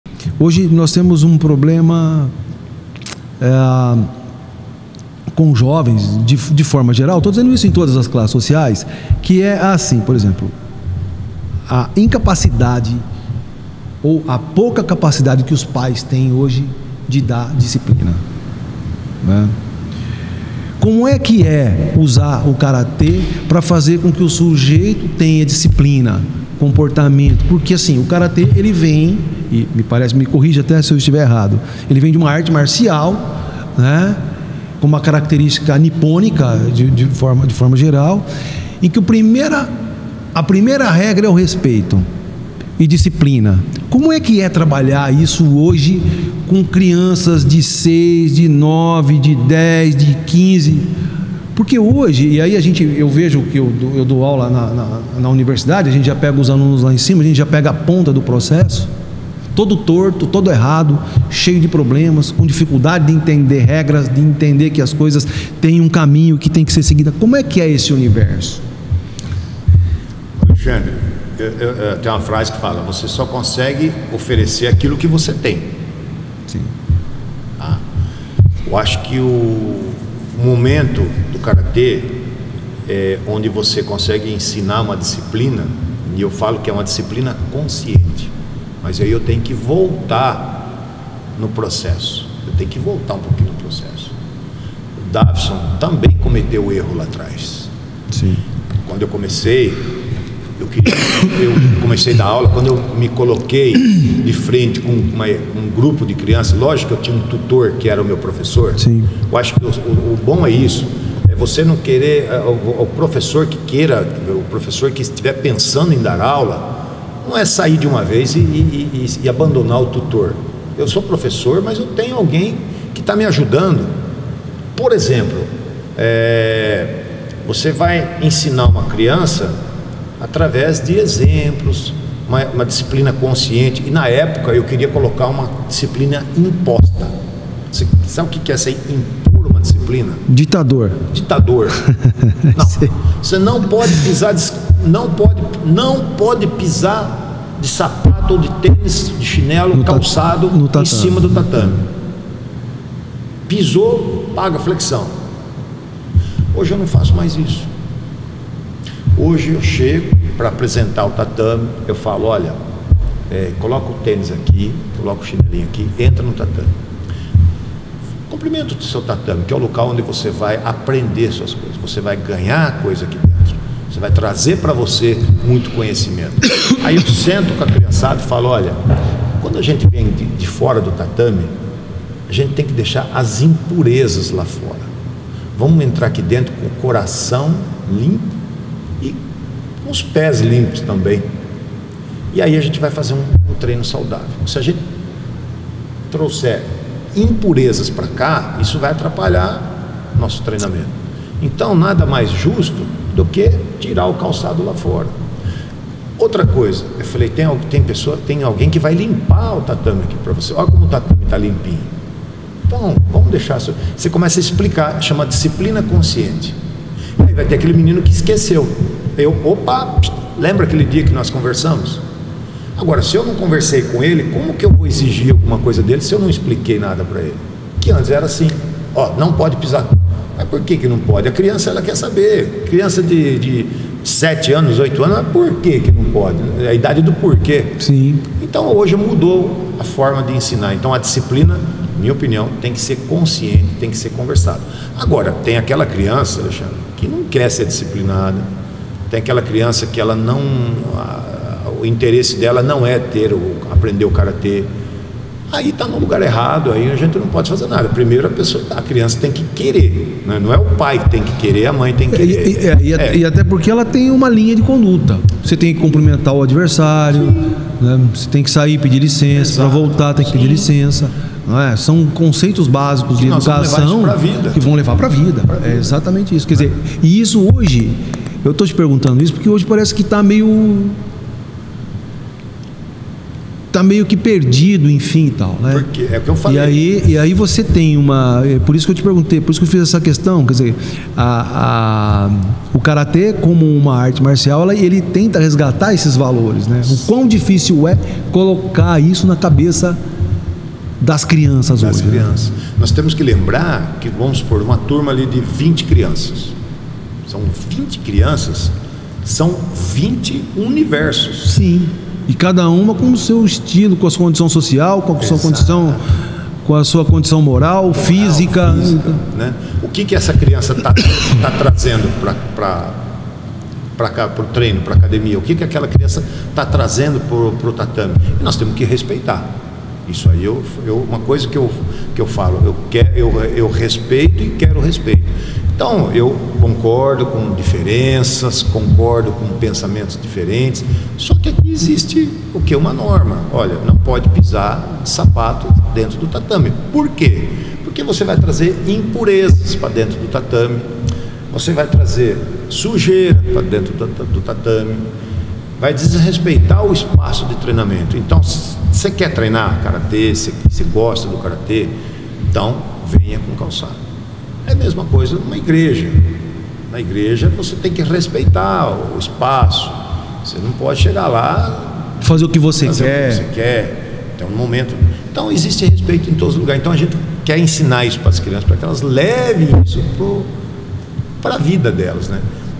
Cortes PodCast Uniara – Davison Tosadori – Karatê – Como trabalha disciplina e respeito
O PodCast Uniara foi até Matão, no Campus da Universidade de Araraquara na cidade vizinha para conversar com o professor de Educação Física e professor de Karatê Davison José Tosadori. Nosso bate papo foi sobre a arte marcial Karatê, originária do Japão, focada no desenvolvimento físico, mental e espiritual. Envolve técnicas de socos, chutes, bloqueios e golpes de mão aberta. Além do aspecto físico, enfatiza valores como disciplina, respeito e autocontrole. As graduações são representadas por faixas coloridas, indicando o nível de habilidade do praticante. Existem diferentes estilos de Karatê, como Shotokan, Wado-ryu e Goju-ryu, cada um com suas ênfases específicas. O treinamento no Karatê visa aprimorar a autodefesa e promover o equilíbrio entre corpo e mente. "Como trabalha disciplina e respeito"

Realização: TV Uniara
Apresentação: Alexandre Pierini
Imagens: Fagner Pereira
Produção: Alexandre Pierini e Fagner Pereira